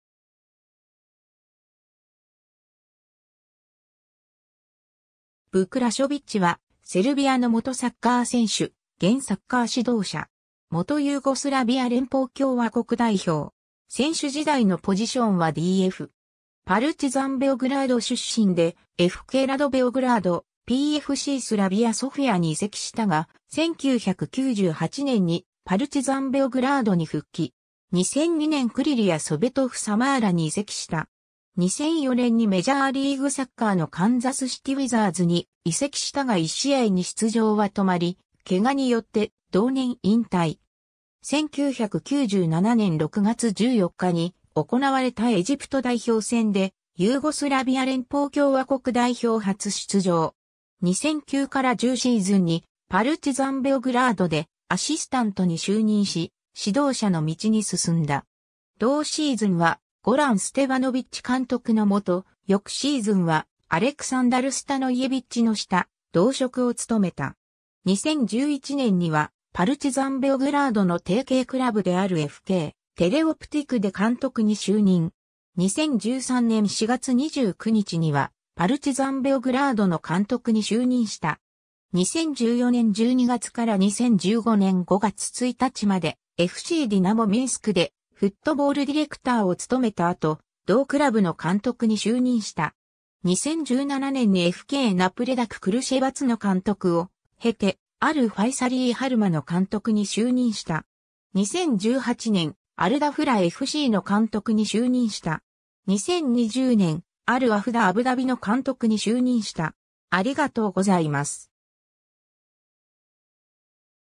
ヴク・ラショヴィッチ
セルビア・モンテネグロのサッカー選手
サッカーセルビア・モンテネグロ代表選手
パルチザン・ベオグラードの選手
FKラド・ベオグラードの選手
PFCスラヴィア・ソフィアの選手
スポルティング・カンザスシティの選手
パルチザン・ベオグラードの監督
FCディナモ・ミンスクの監督
ヴク・ラショヴィッチ（セルビア語: Вук Рашовић, Vuk Rašović、1973年1月3日 - ）は、セルビアの元サッカー選手、現サッカー指導者。
元ユーゴスラビア連邦共和国代表。
選手時代のポジションはDF。
 パルチザン・ベオグラード出身でFKラド・ベオグラード、PFCスラヴィア・ソフィアに移籍したが、1998年にパルチザン・ベオグラードに復帰。
2002年クリリヤ・ソヴェトフ・サマーラに移籍した。
2004年にメジャーリーグサッカーのカンザスシティ・ウィザーズに移籍したが1試合に出場は止まり、怪我によって同年引退。
 1997年6月14日に行われたエジプト代表戦でユーゴスラビア連邦共和国代表初出場。
 2009-10シーズンにパルチザン・ベオグラードでアシスタントに就任し指導者の道に進んだ。
同シーズンはゴラン・ステヴァノヴィッチ監督の下、翌シーズンはアレクサンダル・スタノイェヴィッチの下、同職を務めた。
 2011年にはパルチザン・ベオグラードの提携クラブであるFKテレオプティクで監督に就任。
2013年4月29日にはパルチザン・ベオグラードの監督に就任した。
 2014年12月から2015年5月1日までFCディナモ・ミンスクでフットボールディレクターを務めた後、同クラブの監督に就任した。
 2017年にFKナプレダク・クルシェヴァツの監督を経てアル・ファイサリー・ハルマの監督に就任した。
 2018年、アル・ダフラFCの監督に就任した。
 2020年、アル・ワフダ・アブダビの監督に就任した。